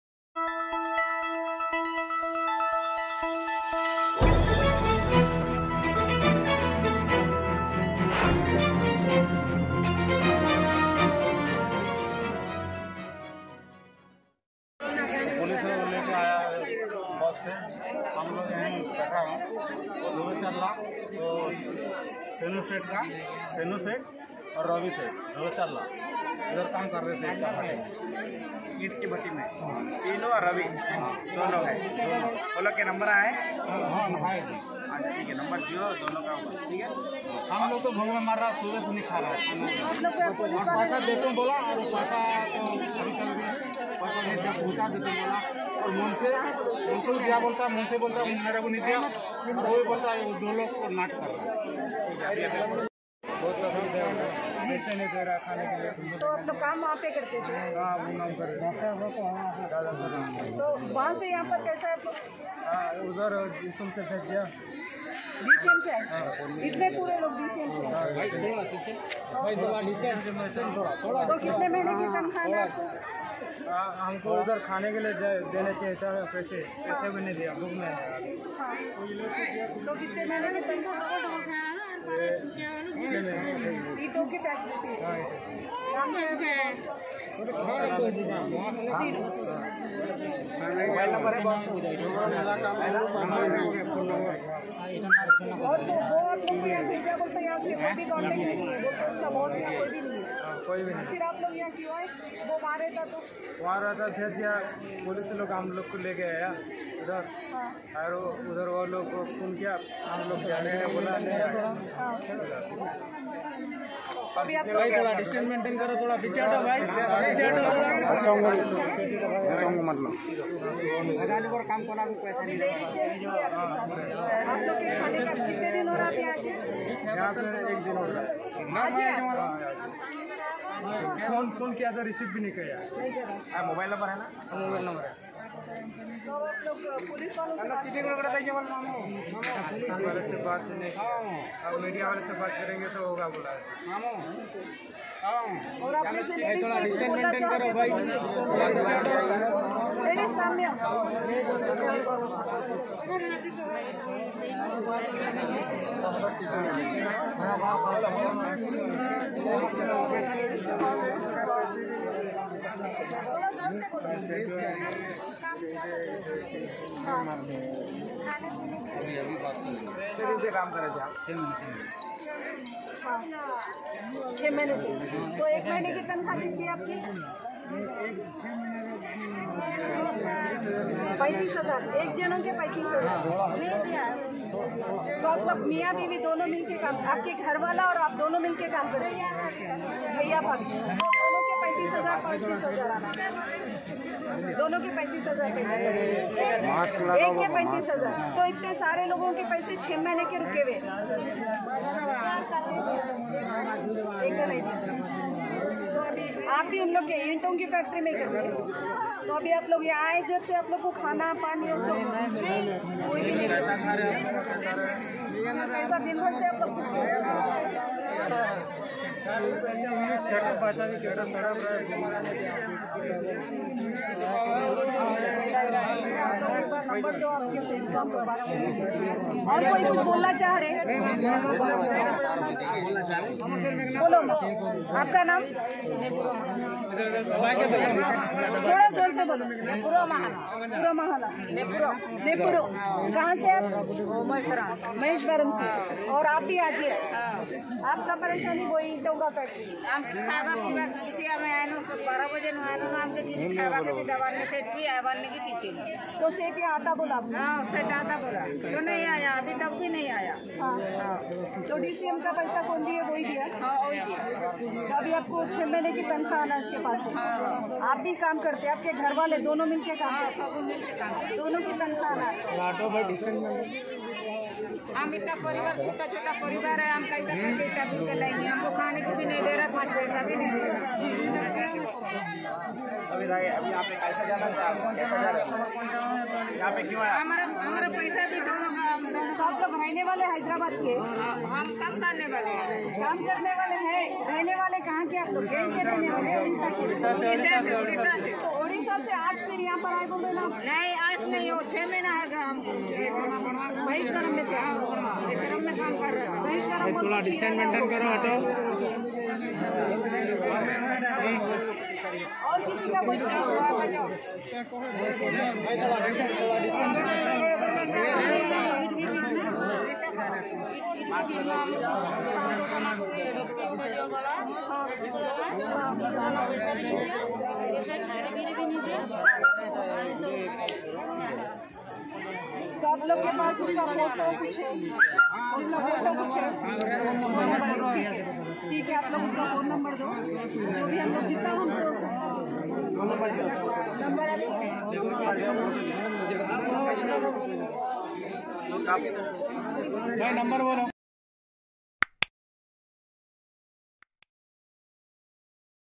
Migrant Workers From Other States Facing lot of Problems Due to lockdown - Interview with them
Migrant Workers From Other States Facing lot of Problems Due to lockdown - Interview with them by Safiya Mahi, Freelance Journalist & Social Worker.